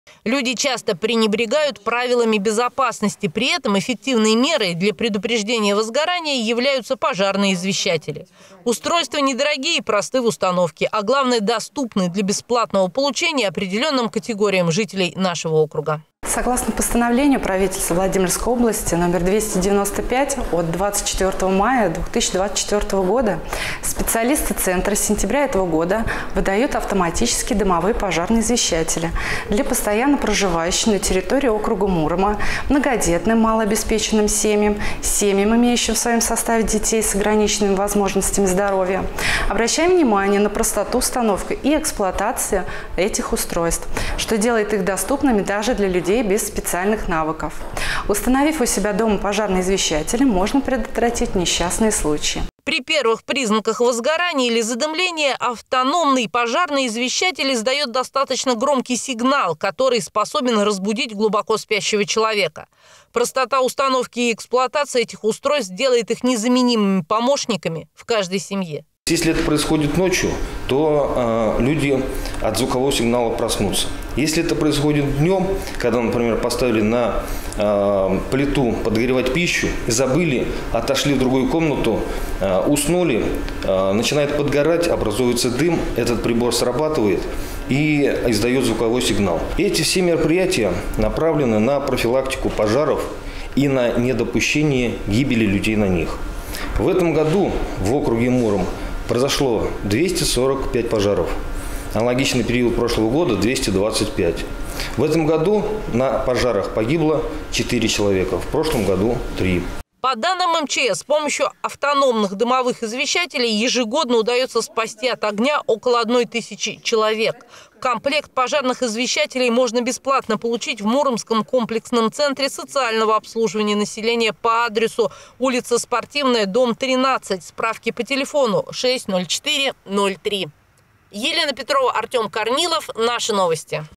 Пожарные извещатели
Установка пожарных извещателей стала хорошей дополнительной мерой поддержки для многодетных малообеспеченных семей и семей, воспитывающих детей с ограниченными возможностями здоровья. Прибор реагирует на дым на стадии возгорания, когда потушить огонь ещё можно подручными средствами.
Комплект из трёх извещателей вместе с батарейками можно получить бесплатно в Муромском комплексном центре социального обслуживания населения".